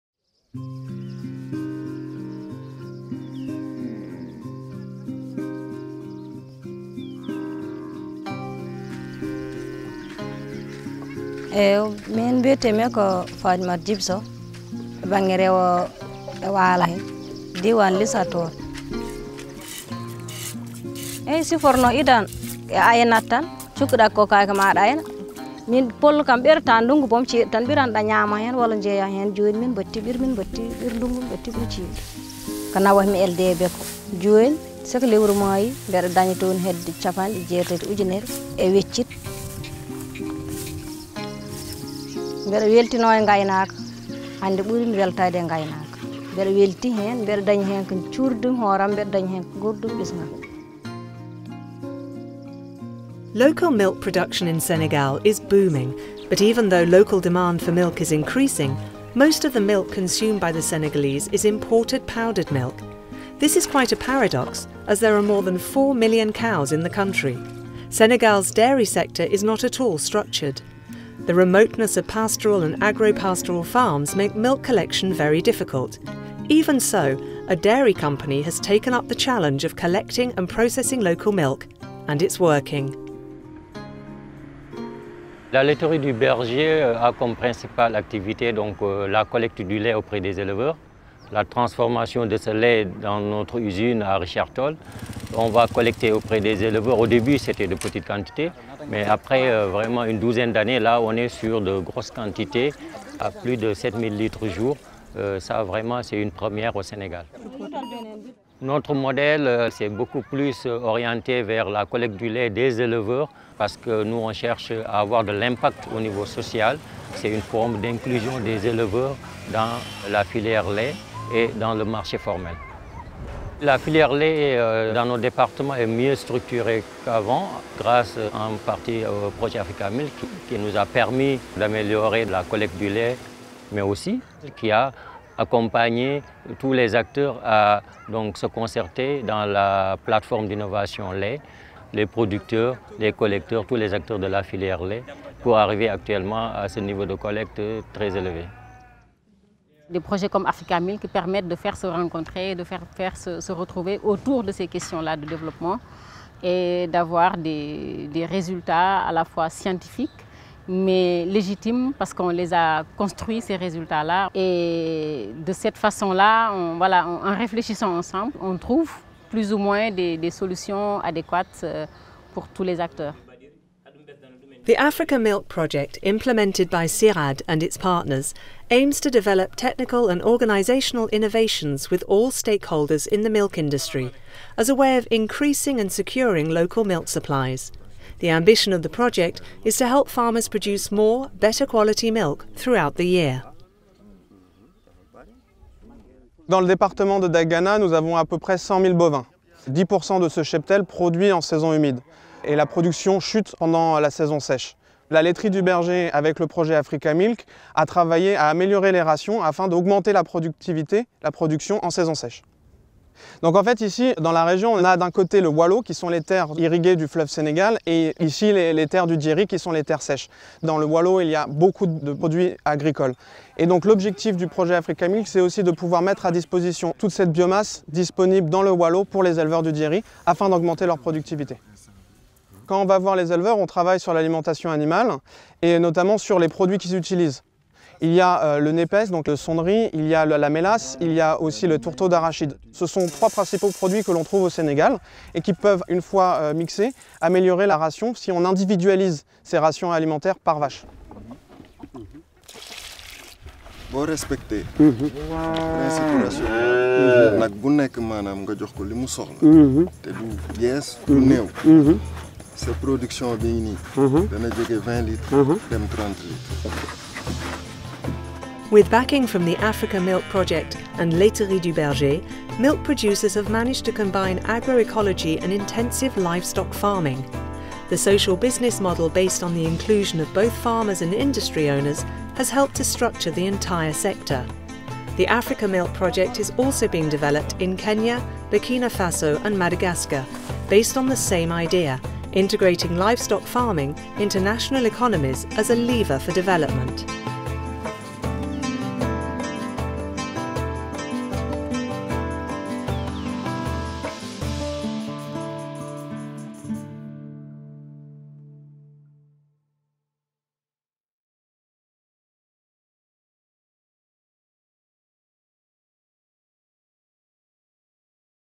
An improved and better structured milk sector
Demand for dairy products is currently increasing in Africa. However, dairies have problems sourcing local milk in terms of volume, regularity and quality. The Africa-Milk project supports the co-design and implementation of technical, organizational and institutional innovations to increase and secure local milk supplies in Senegal, Madagascar, Burkina Faso and Kenya.